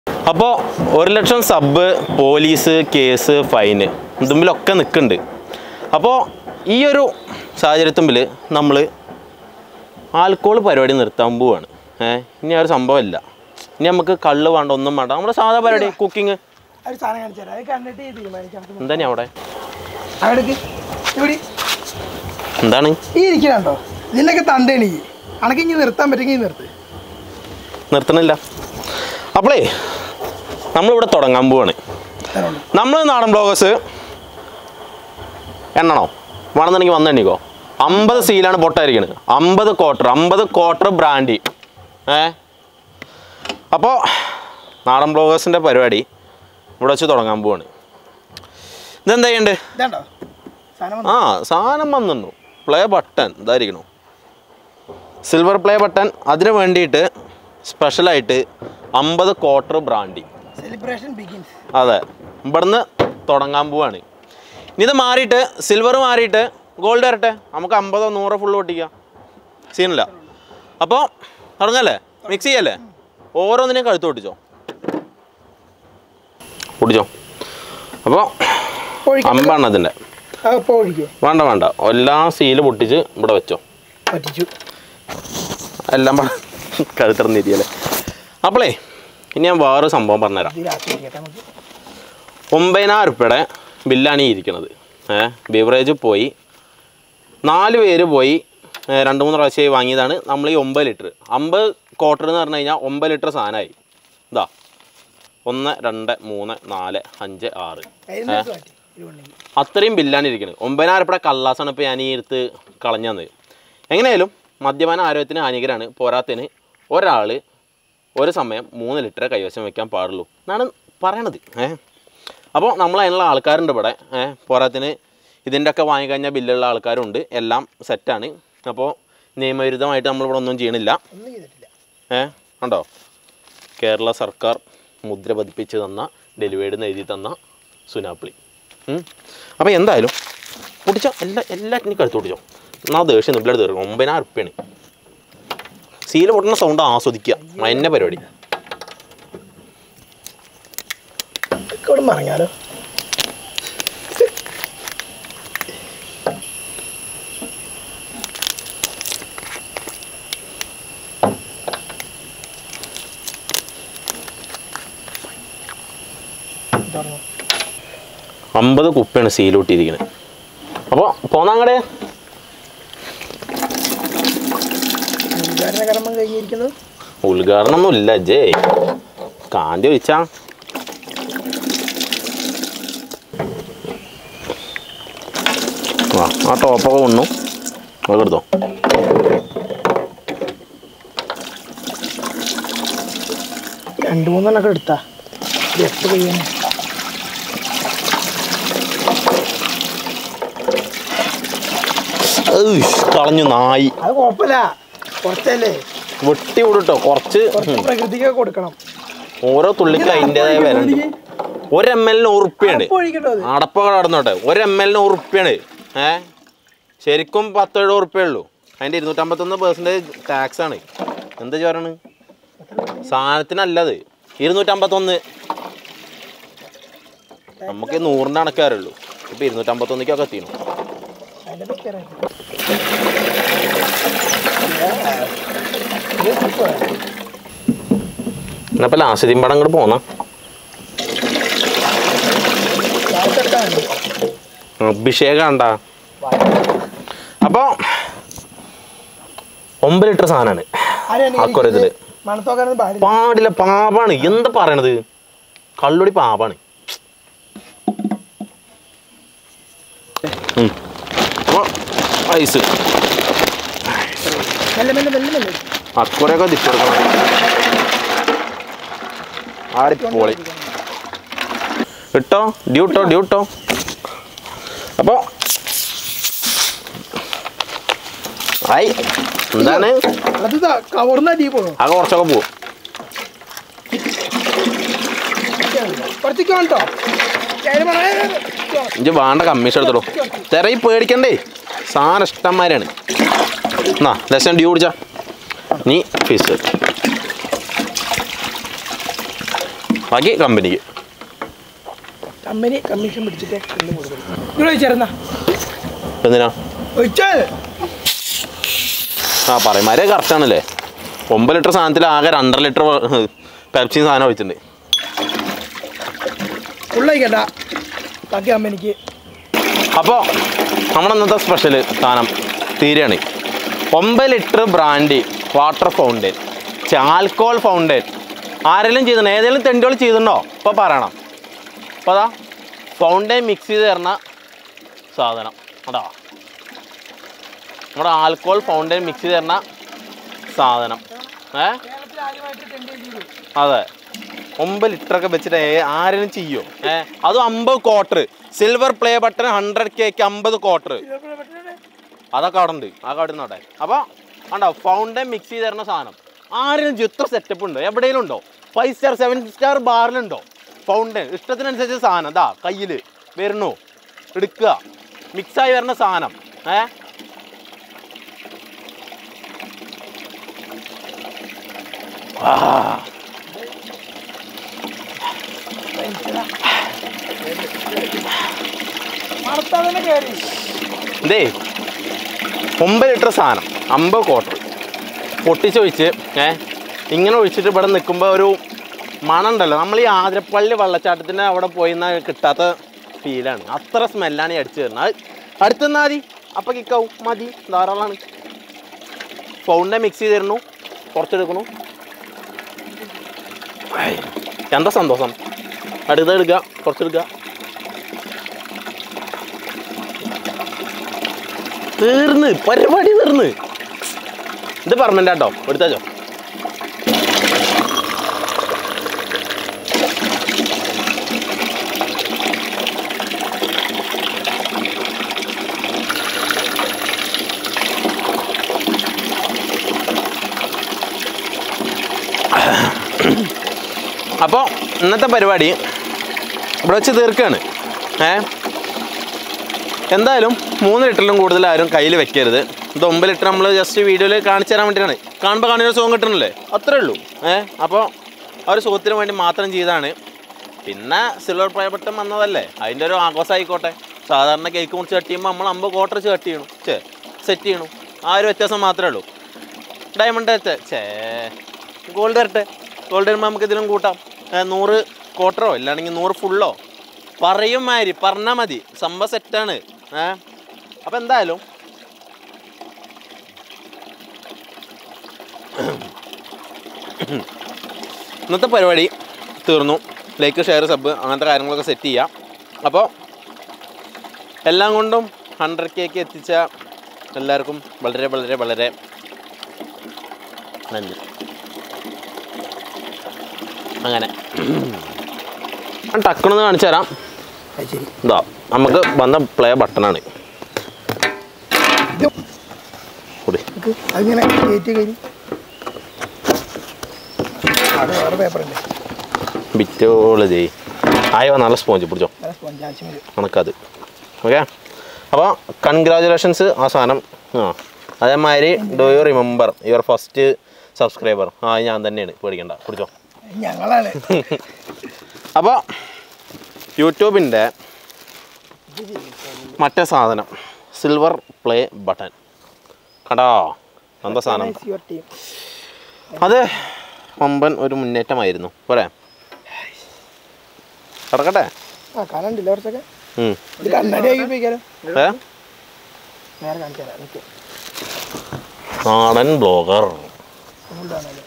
50 Quarter Brandy For 100k Celebration | Biggest 100k Celebration | 50 Kottar Alcohol Fountain
palakkadan village boys making cocktail brandy at our river side